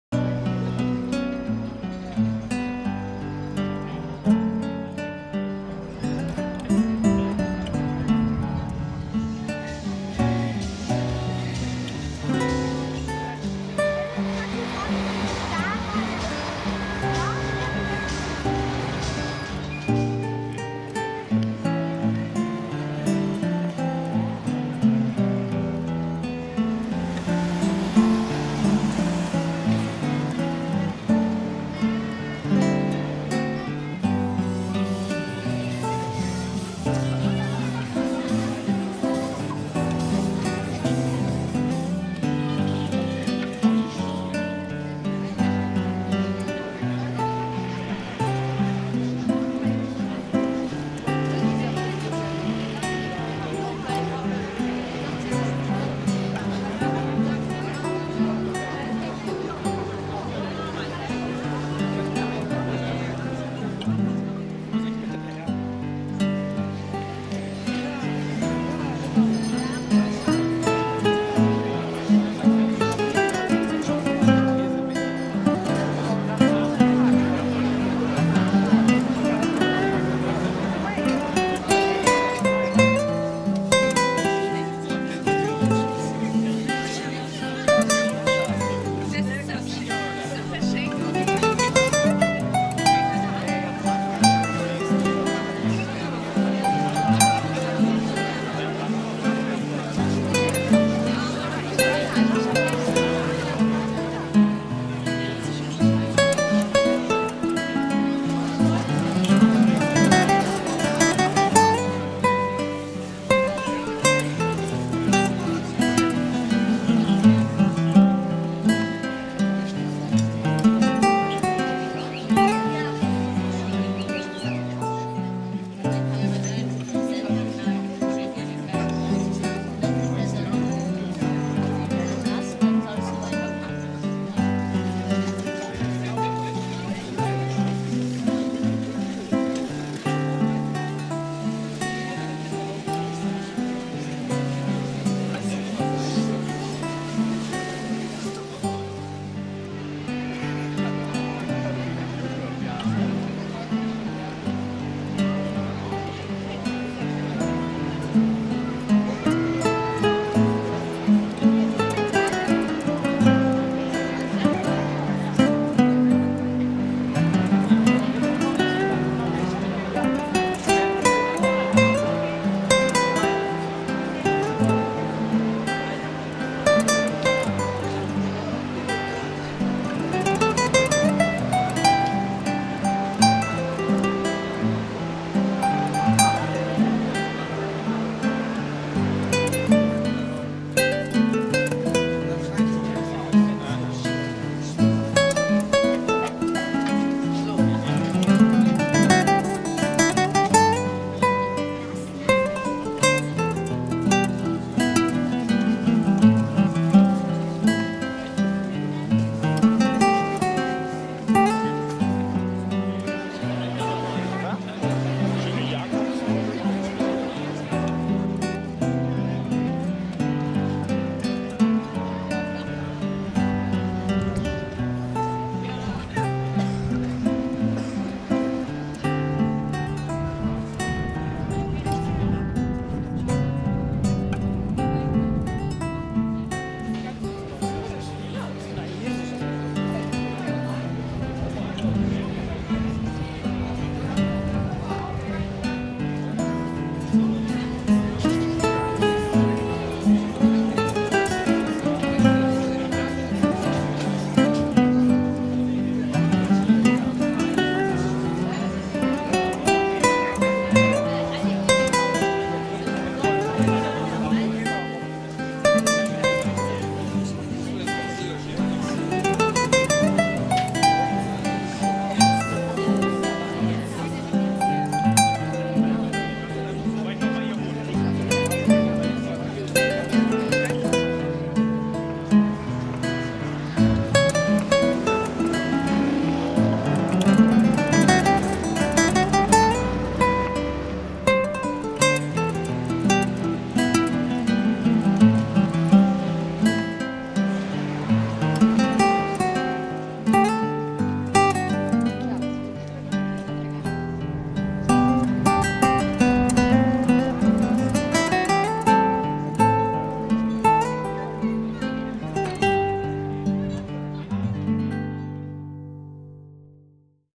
Weihnachten in Hamburg (Christmas in Hamburg/Germany)
Ein Bummel über die Hamburger Weihnachtsmärkte
(Kamera Panasonic HDC-SD909)